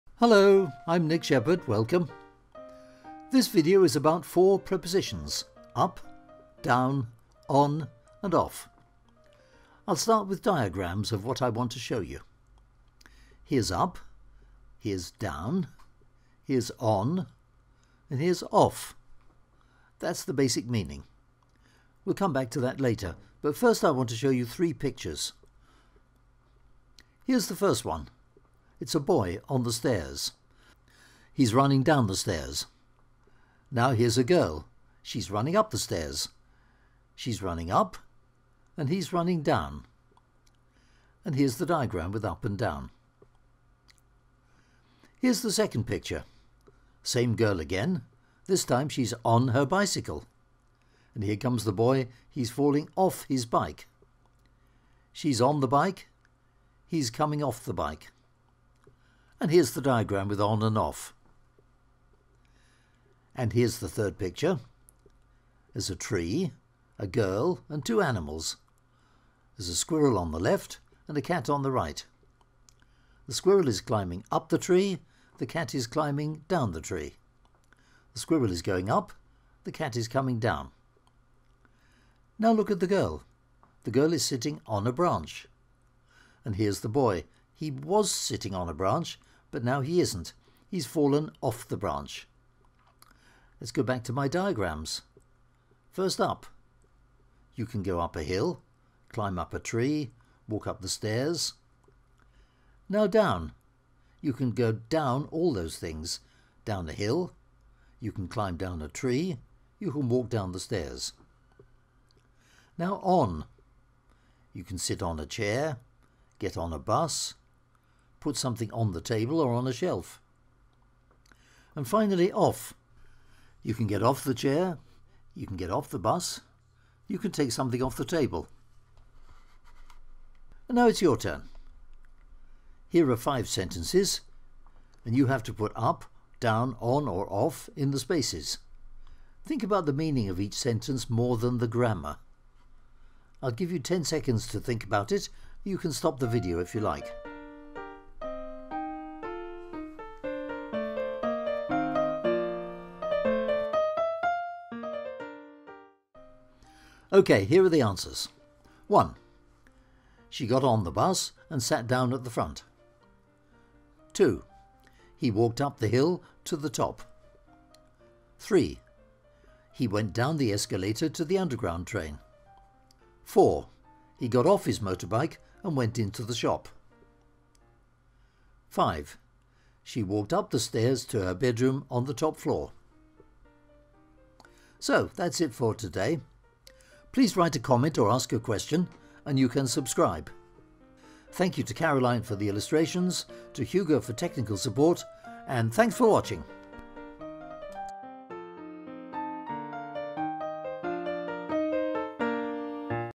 up down on off
This is a remake of a video about four prepositions: on, off, up, down.

And here's a link to some expert notes on this topic:

I hope this video is better than the earlier one; I've added in more diagrams, and I've tried to make it clearer and more concise.

You can watch the video with captions, translation in your language (beta) and in high definition.